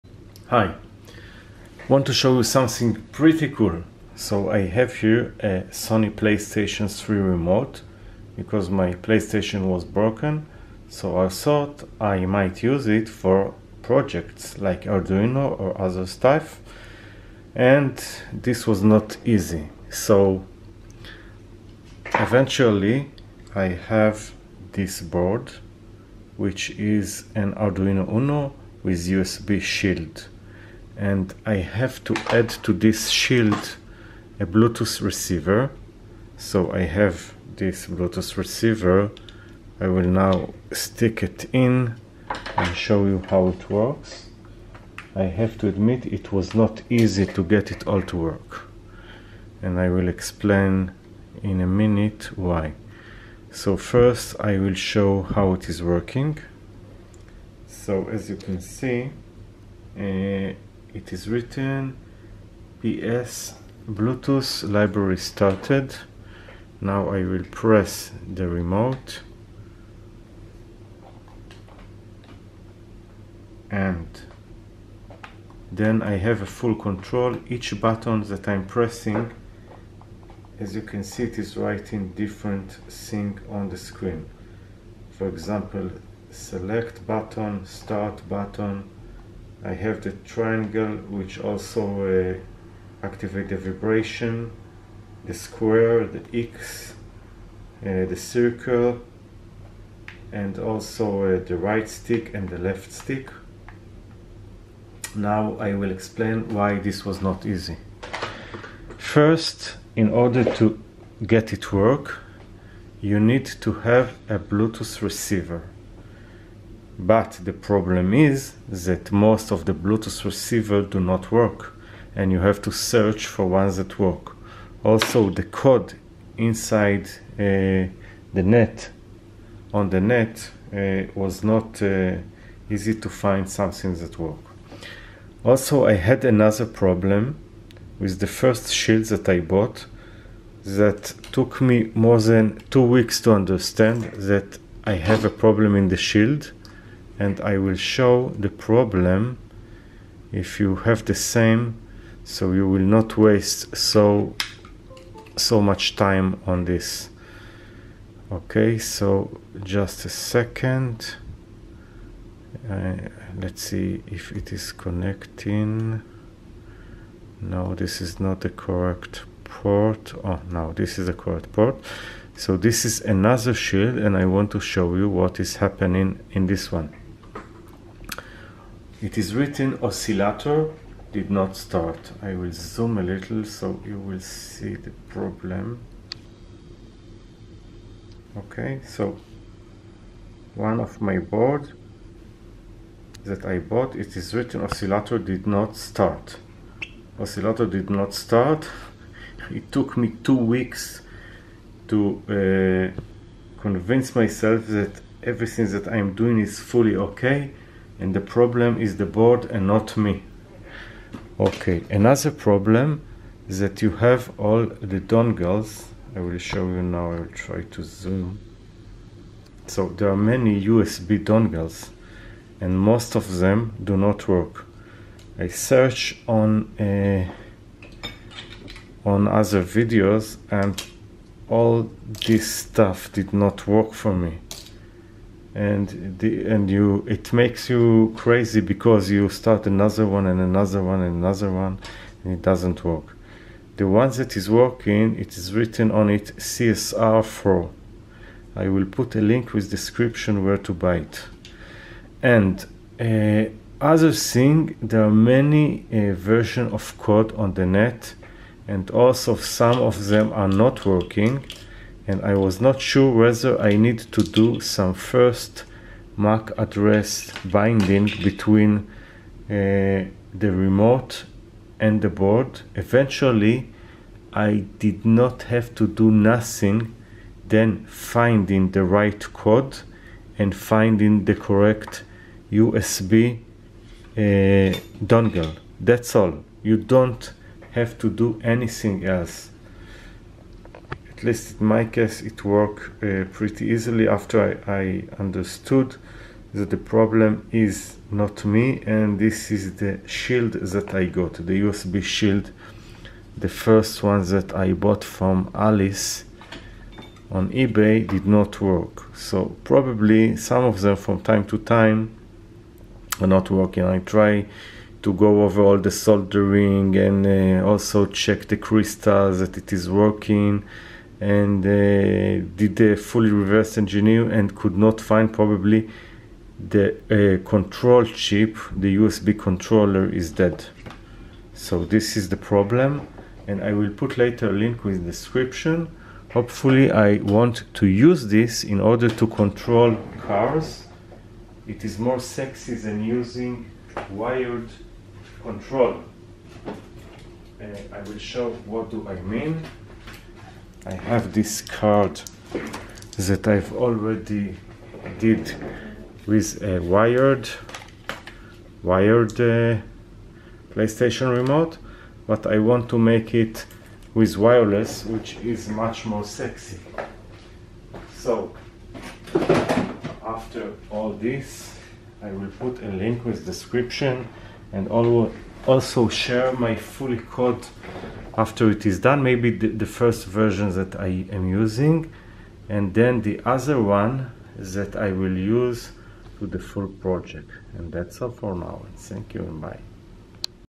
Connecting PS3 to Arduino
This is a short description how to connect PS3 Remote to Arduino
Basically what you need is these things:
1. PS3 remote
2. USB shield for Arduino
4. The correct Code to use